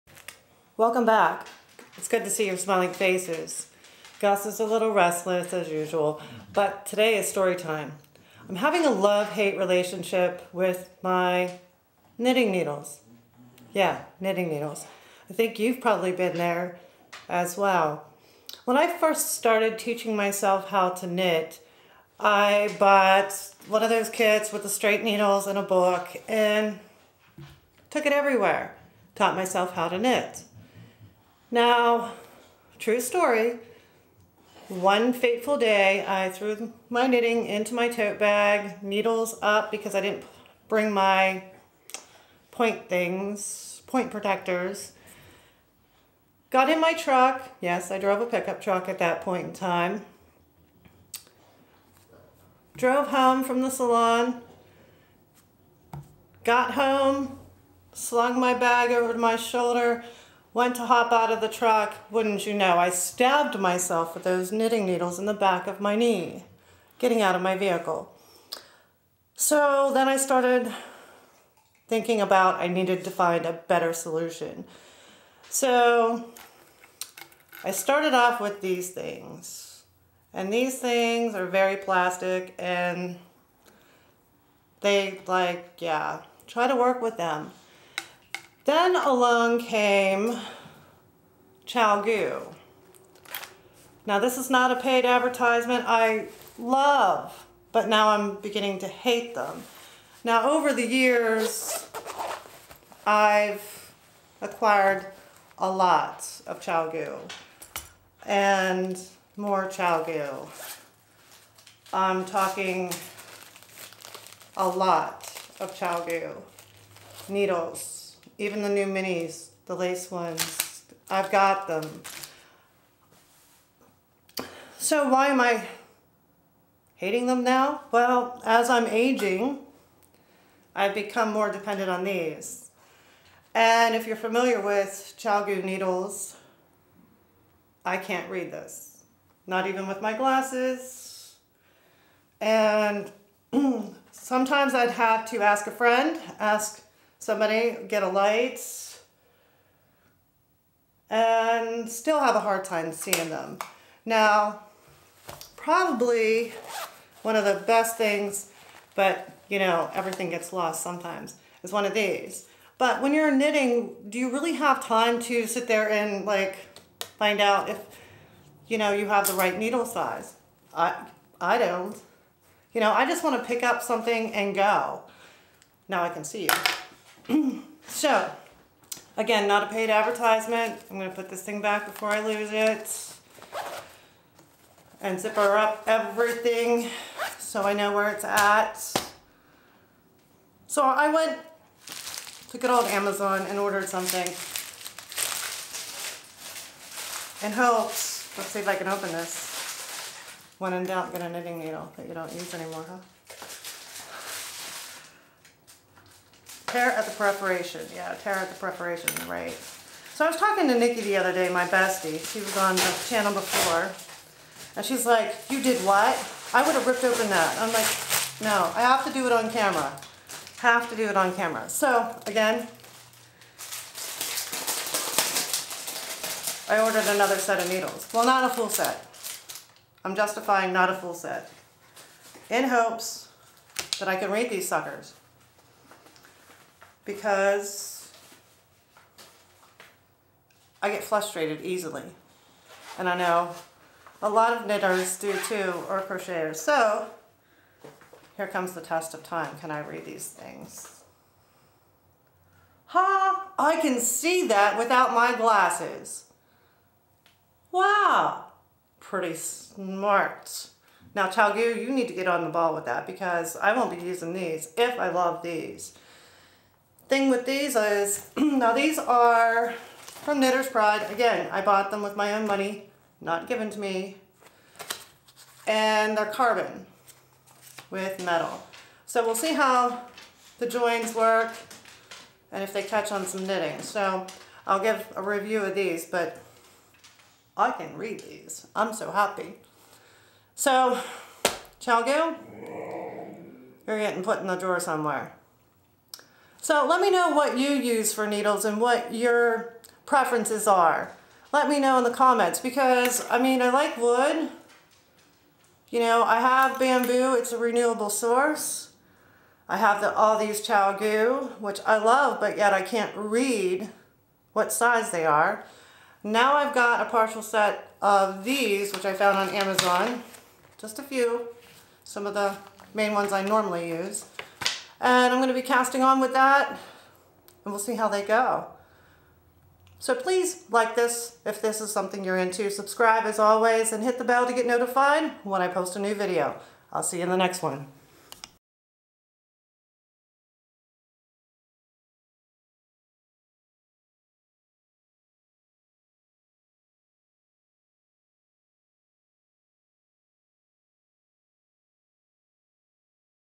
Love and Hate Relationship with... My Knitting Needles!
The never ending search for the perfect tools to create with, beginning with my current love/hate relationship with the tools I'm using to create with.